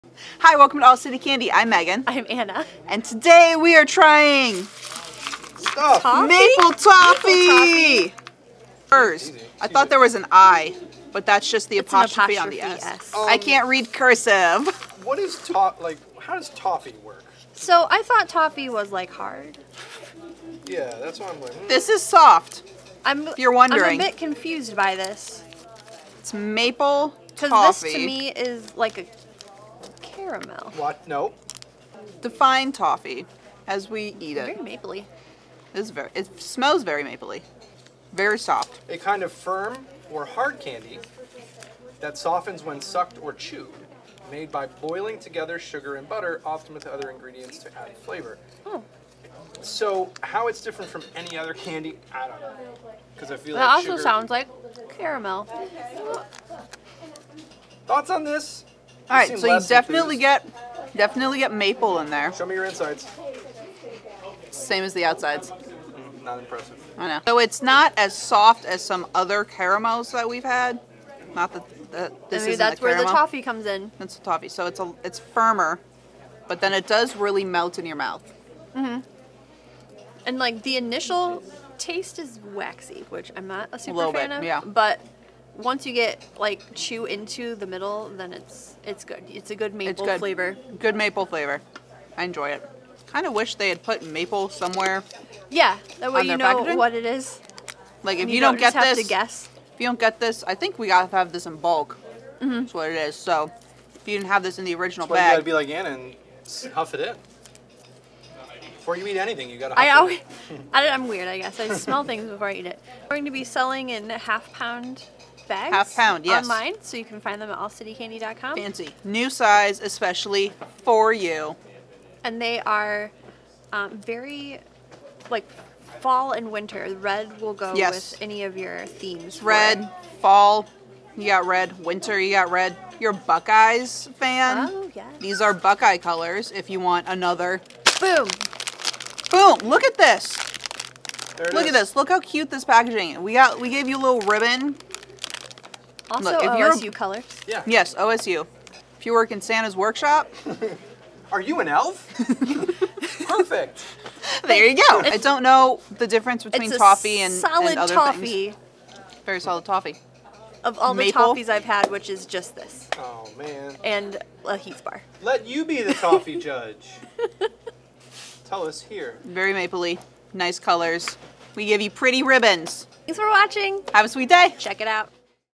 The Taste Of Canada - Kerr's Maple Kisses Taste Test - All City Candy Unwrapped
This soft creamy toffee is made with 10% real Canadian maple syrup.  Individually wrapped to make it easy to share! Take them with you on the go or display them in a candy bowl.